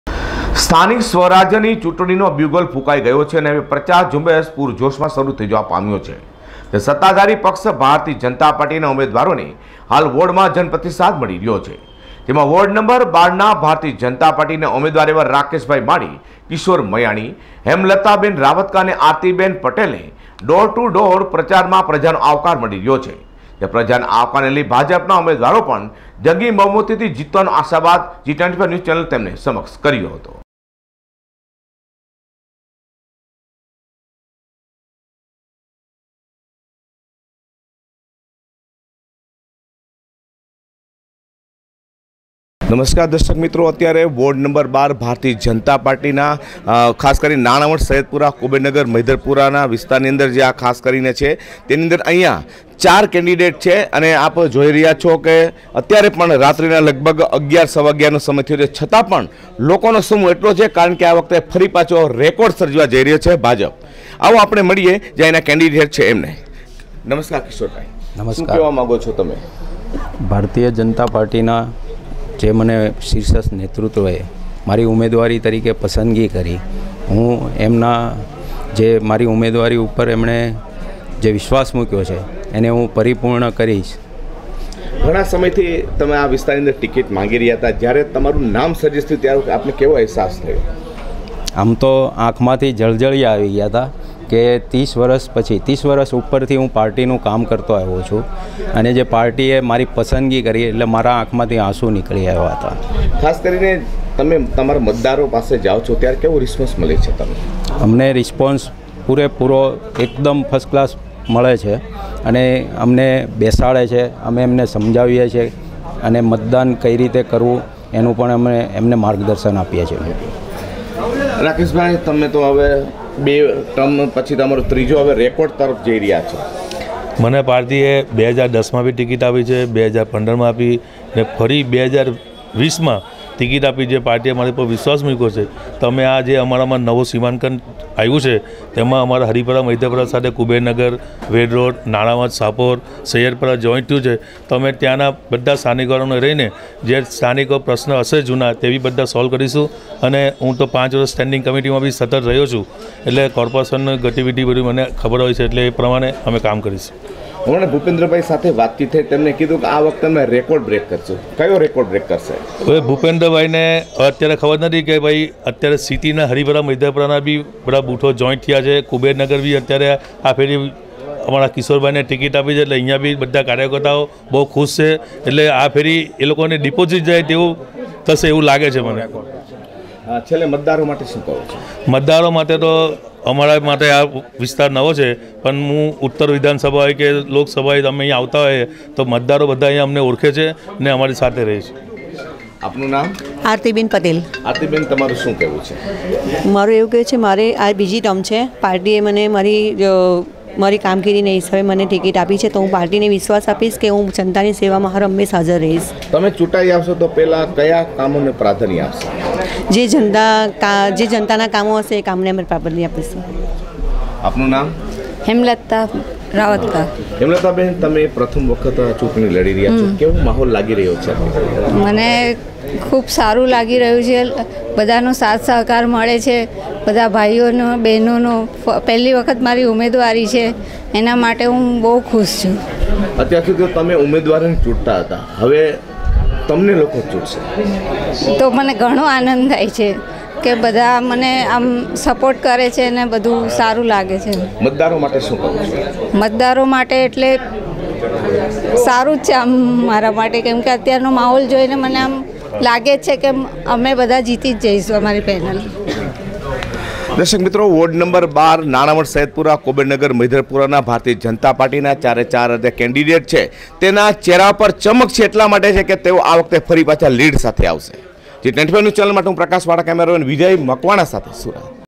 BJP સ્થાનિક સ્વરાજ્યની ચુંટણીમાં ભાજપના ઉમેદવારોને પ્રજાનો બહોળો પ્રતિસાદ | G 24 NEWS
વોર્ડ નં. 12ના ભાજપના ઉમેદવારોએ પ્રજાના મળતા આવકારને લઈ જીતનો આશાવાદ વ્યક્ત કર્યો